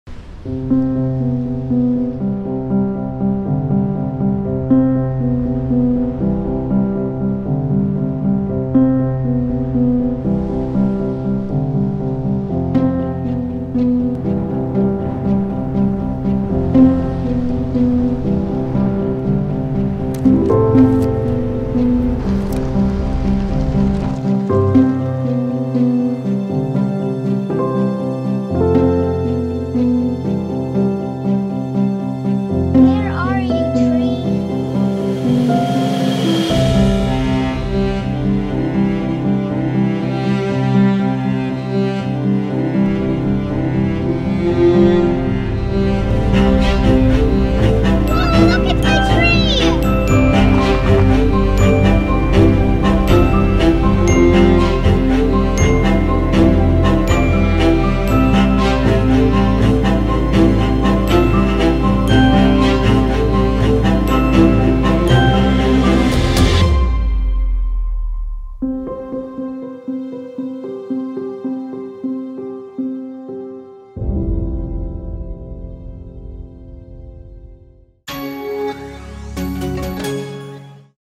The Christmas Tree: Yoga Book
This Christmas holiday, reimagine your world with the power to create and digitize those all-important memories. Because they’re not just sketches, they’re part of your story.

For more info about the Lenovo Yoga Book, and follow YogaBook.